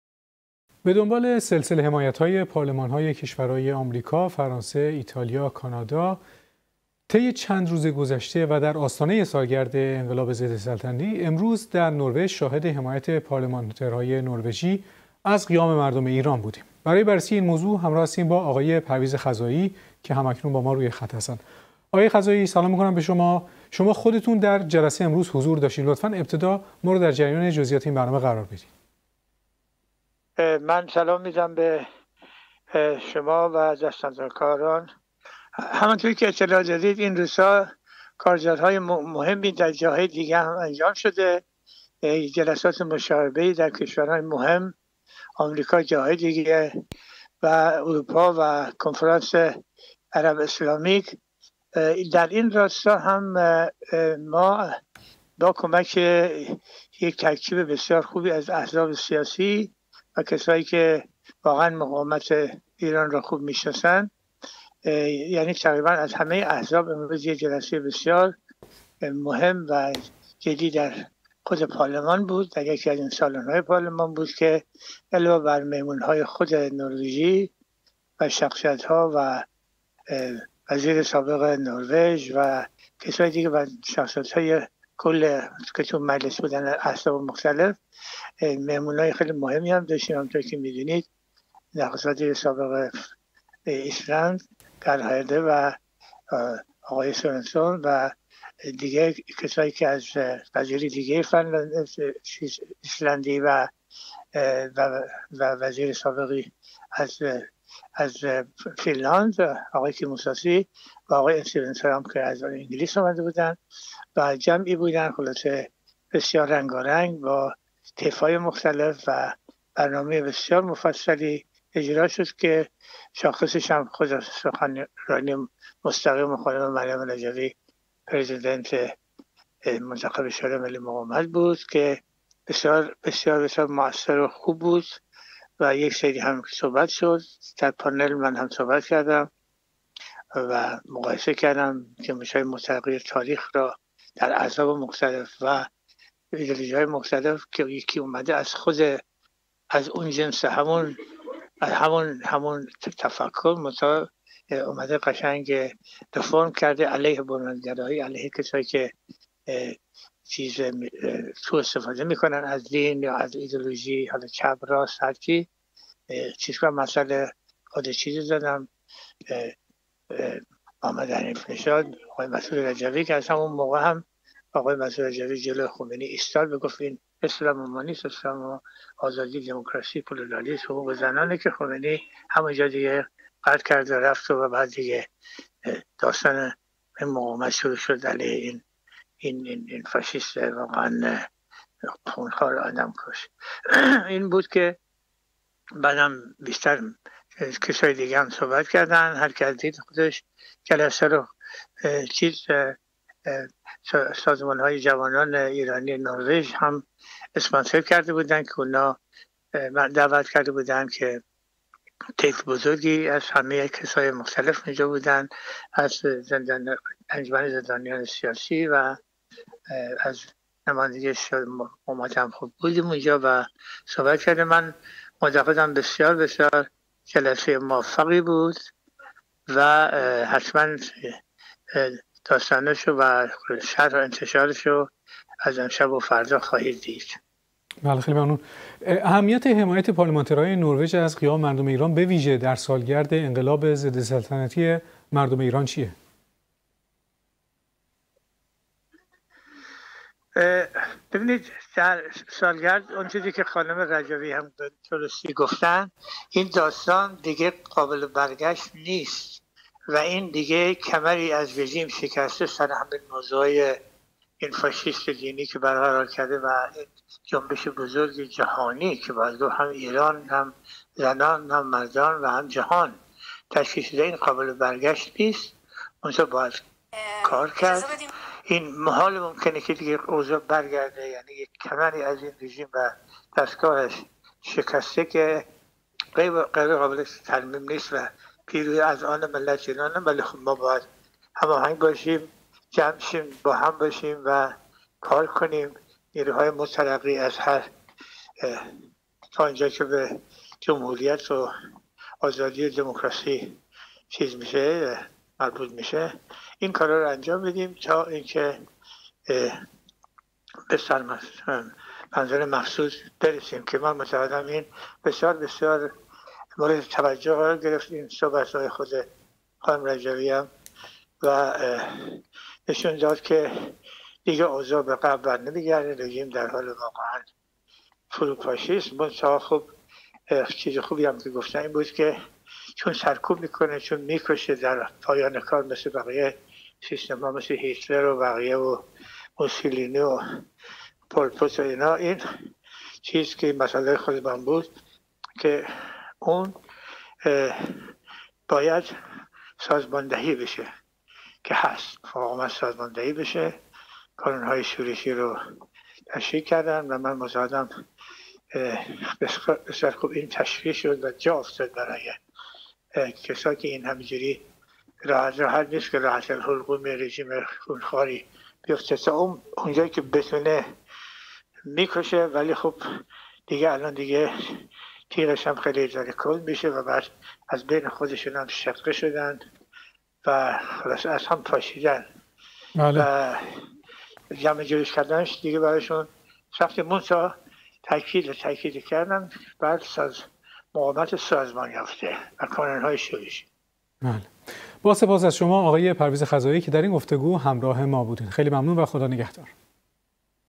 اهمیت حمایت پارلمان نروژ از قیام ایران گفتگو با آقای پرویز خزایی
اهمیت حمایت پارلمانترهای نروژ از قیام مردم ایران، بویژه در سالگرد انقلاب ضدسلطنتی مردم ایران، چیست؟
گفتگو با آقای پرویز خزایی